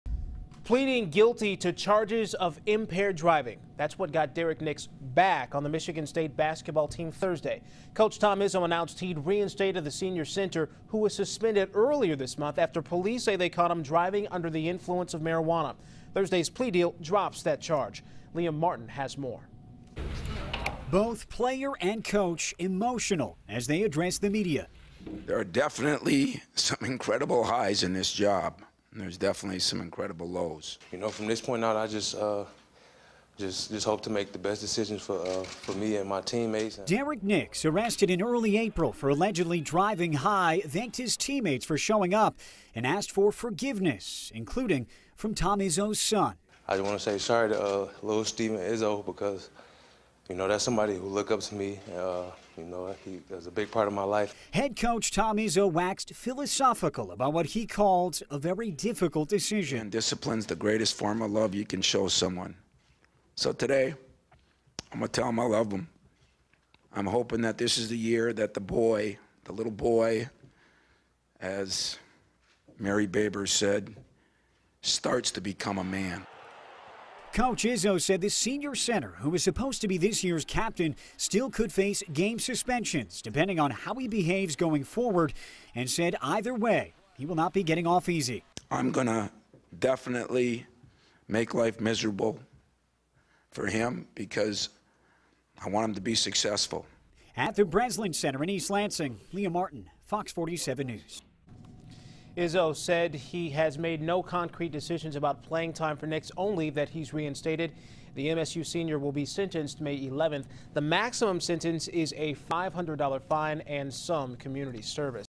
Tearful Nix Announces He Will Remain with MSU Basketball Team
Announcement comes just hours after the star player plead guilty to reduced charges stemming from an April 3 arrest on marijuana charges.